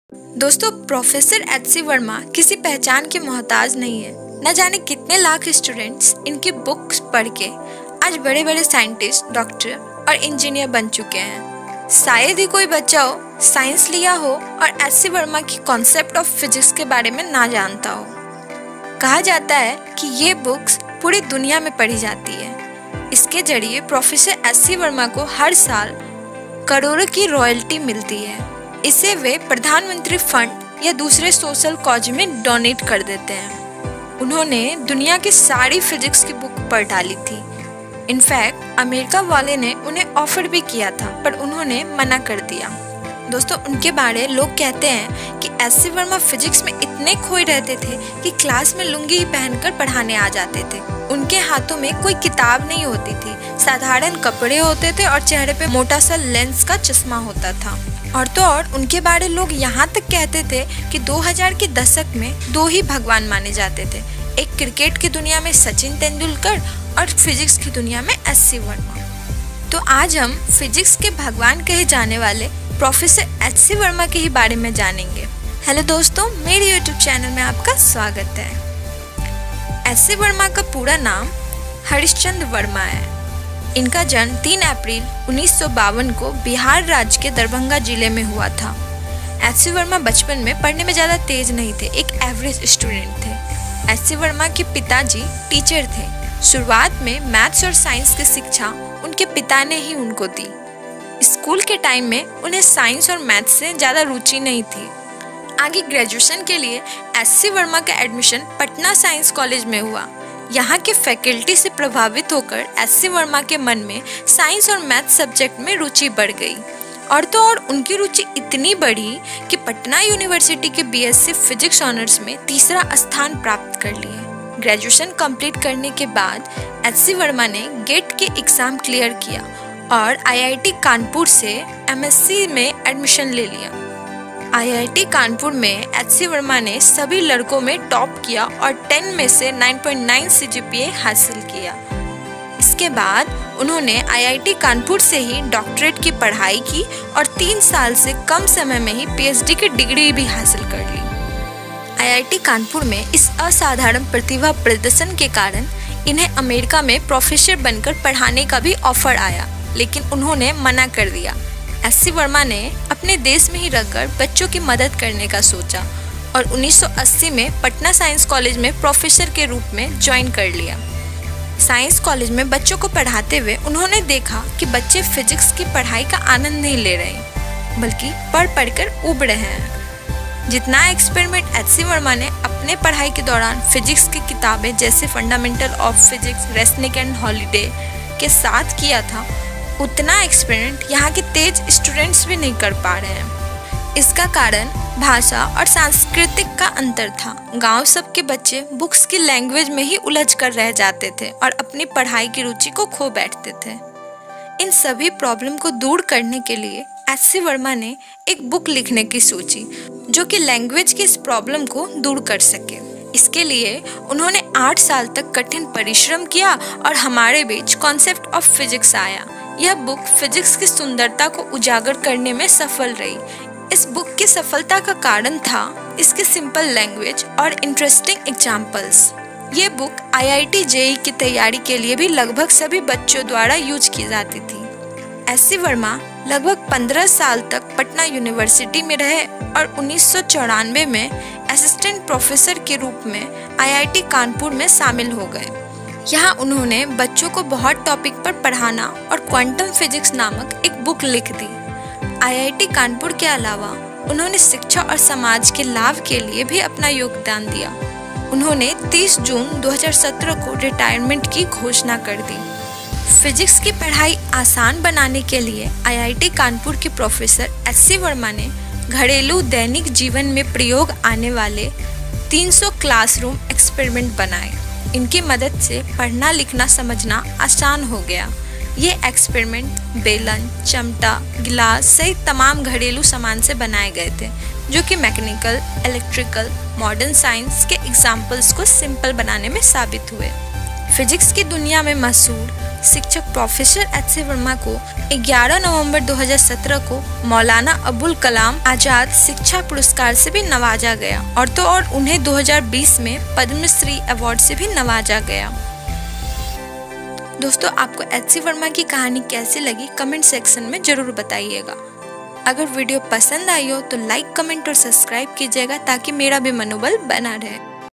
Sucsess Story Of HC Verma | Concept Of Physics | Motivational Story
Success Story Of  HC Verma | HC Verma (born 3 April 1952) is an Indian experimental physicist and a retired professor at the Indian Institute of Technology Kanpur since 1994.